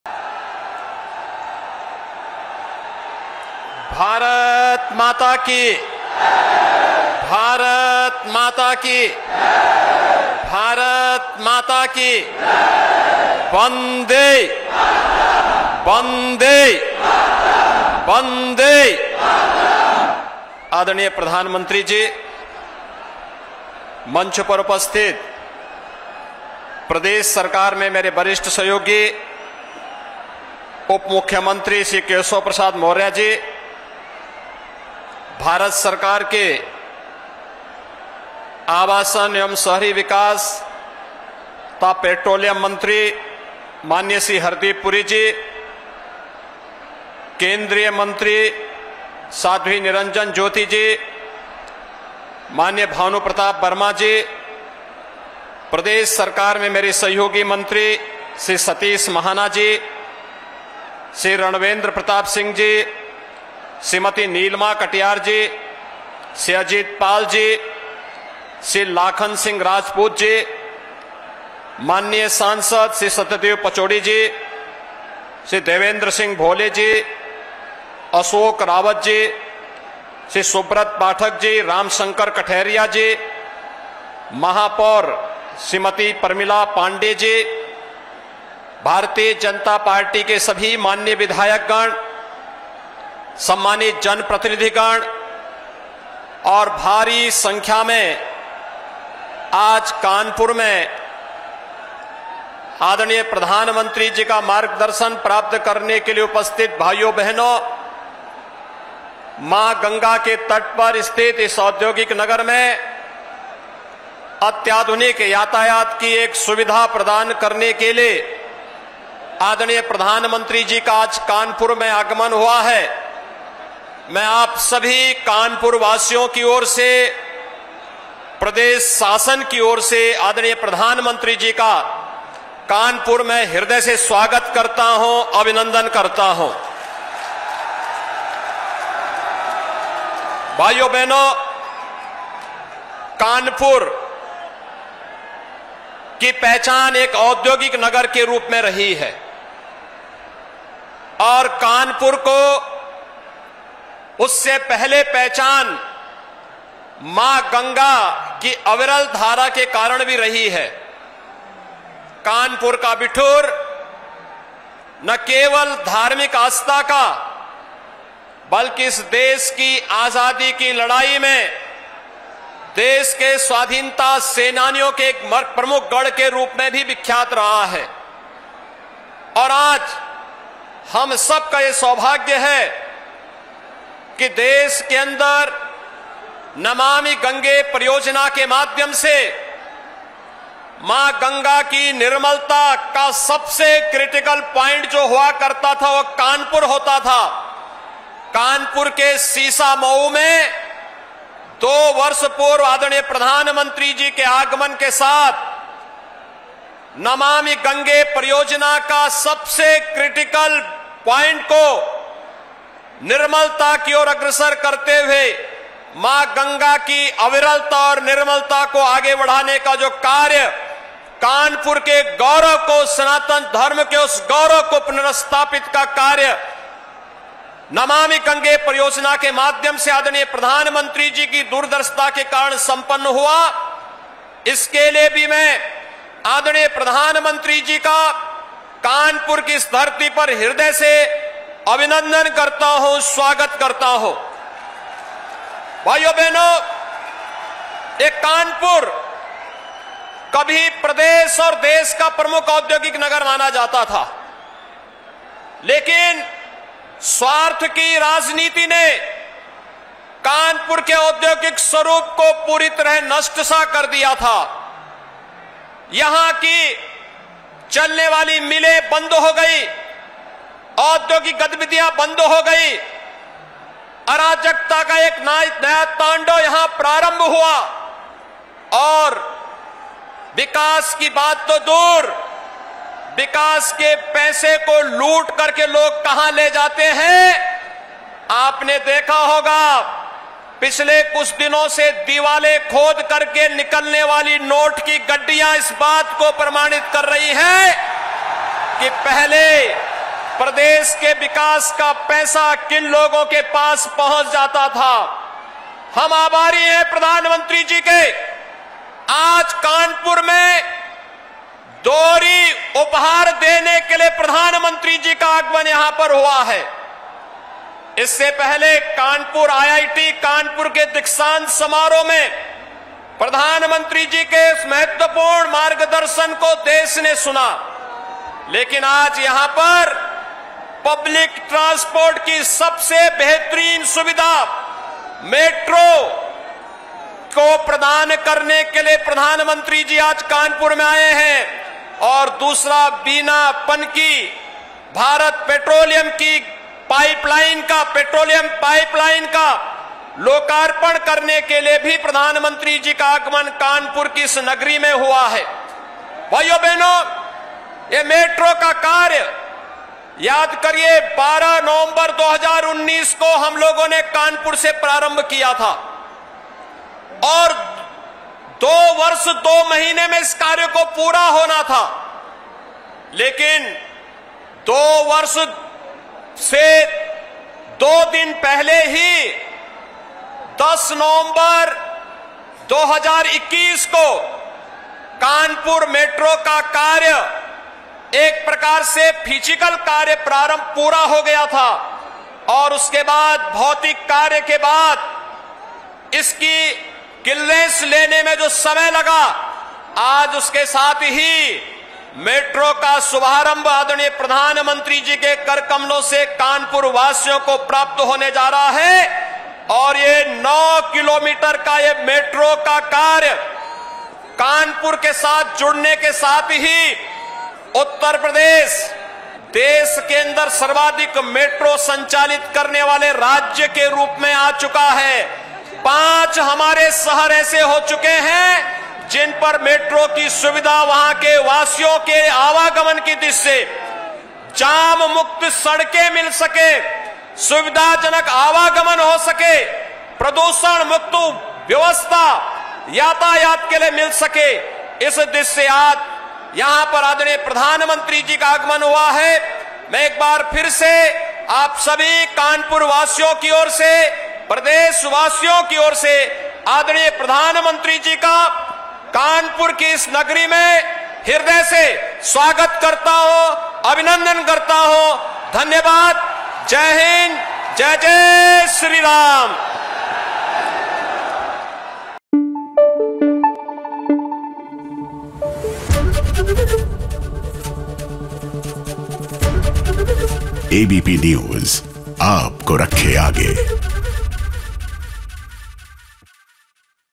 Kanpur Metro के साथ Uttar Pradesh देश में सबसे ज्यादा मेट्रो संचालित करने वाला राज्य बना: CM Yogi
प्रधानमंत्री नरेंद्र मोदी ने कानपुर मेट्रो रेल परियोजना के पूर्ण खंड का उद्घाटन किया और मेट्रो की सवारी की. इस दौरान उत्तर प्रदेश के मुख्यमंत्री योगी आदित्यनाथ भी मौजूद रहे.